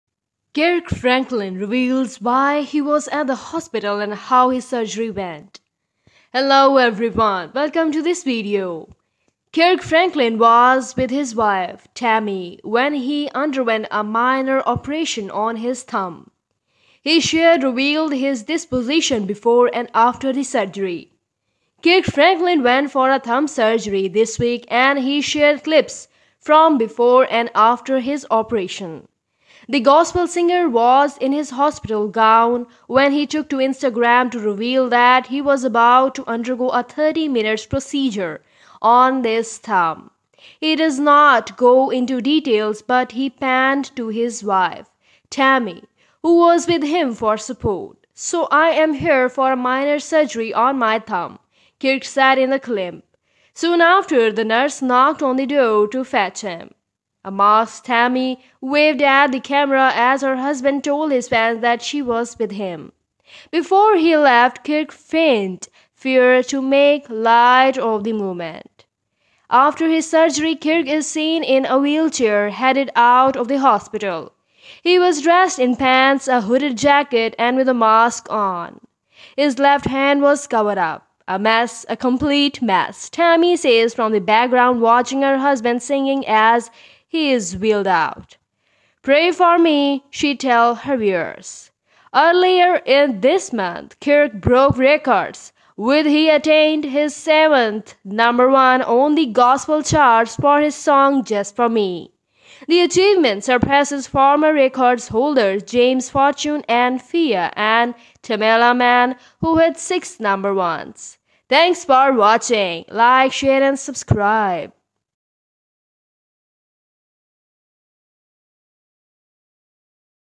Sad News! Gospel Singer Kirk Franklin Hospitalized For Emergency Surgery...Sad Details Inside
Kirk Franklin was with his wife, Tammy when he underwent a minor operation on his thumb.he shared revealed his disposition before and after the surgery.Sad News! Gospel Singer Kirk Franklin Hospitalized For Emergency Surgery...Sad Details Inside.
Kirk Franklin went for a thumb surgery this week and he shared clips from before and after his operation.
The gospel singer was in his hospital gown when he took to Instagram to reveal that he was about to undergo a 30-minute procedure on his thumb. He didn’t go into detail but he panned to his wife, Tammy who was with him for support.
“So I’m here for a minor surgery on my thumb,” Kirk said in the clip. Soon after, the nurse knocked on the door to fetch him.